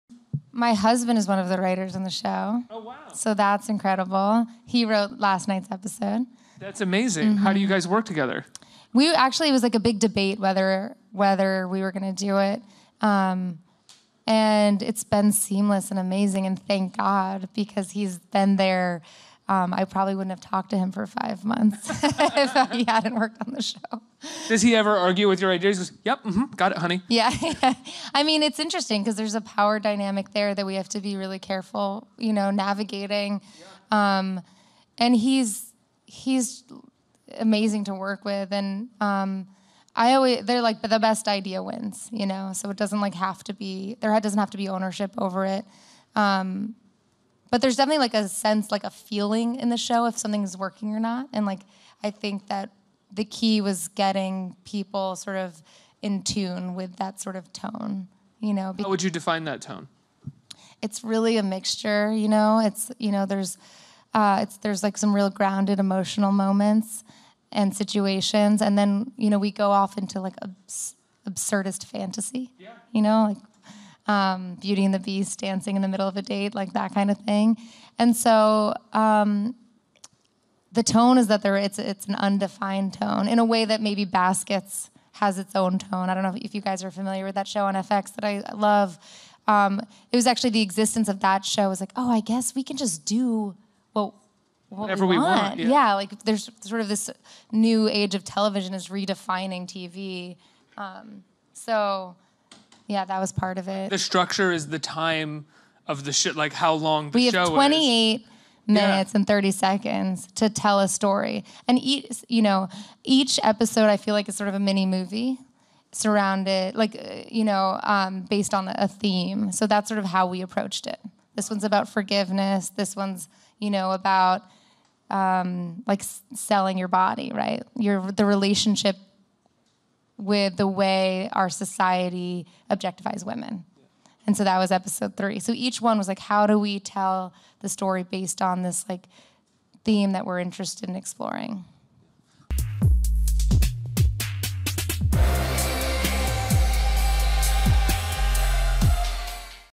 How Frankie Shaw And Her Husband Work Together On “SMILF”
“SMILF” creator and star, Frankie Shaw, describes how her husband fits in as a co-writer on her show.